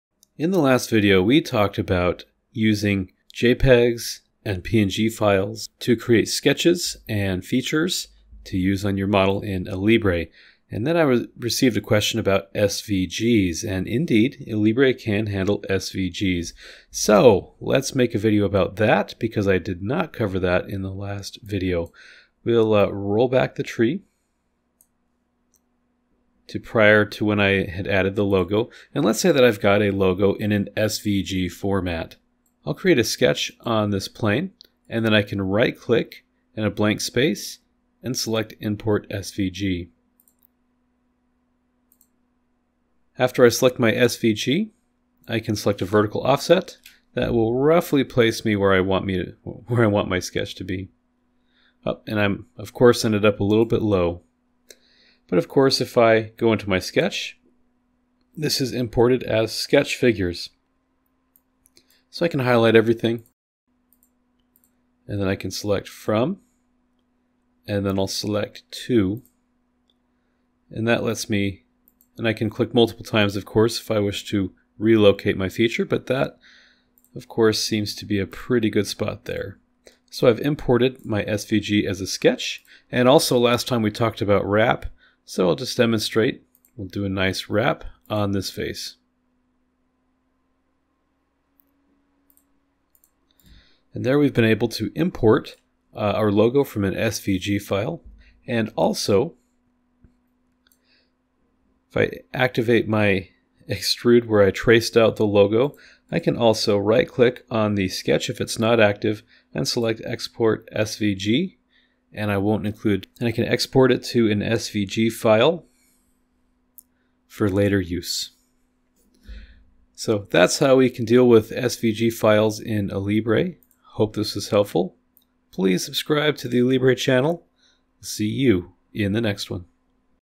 Alibre | Import SVG Files for Sketches and Logos
SVG files can be imported as sketch elements into Alibre.  This allows for the creation of features directly on the model.

The thumbnail is dedicated to my beautiful wife who is an ardent fan of The Lord of the Rings.